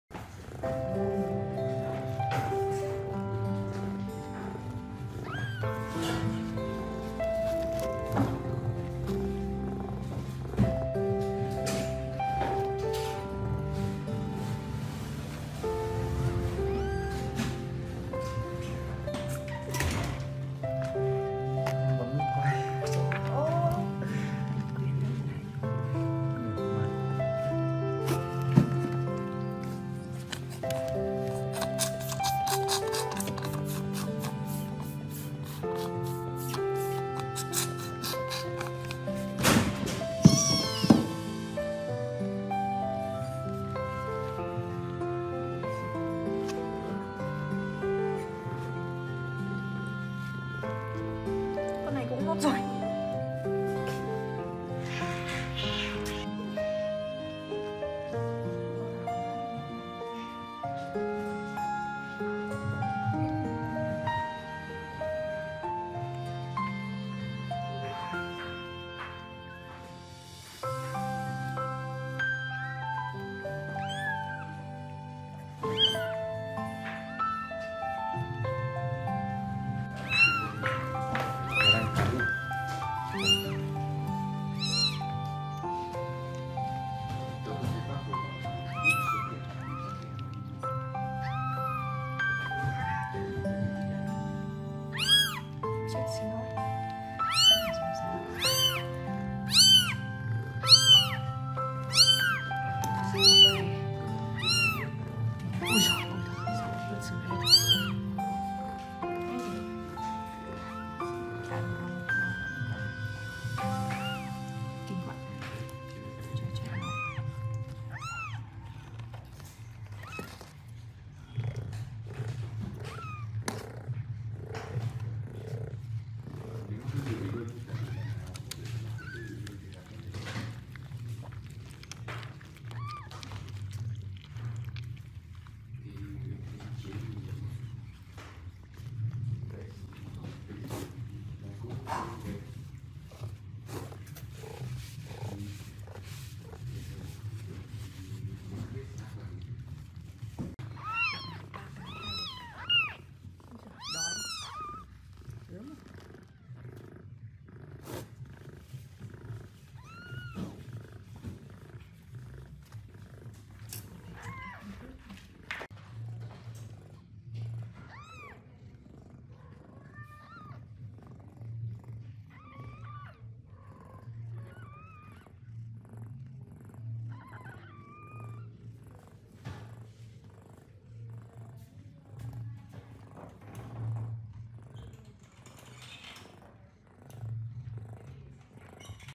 Meow Kittens : Mother cat's first food after giving birth to kittens.
Mother cat's first food after giving birth to kittens.
© Copyright by MeowKittens Production ☞ Do not Reup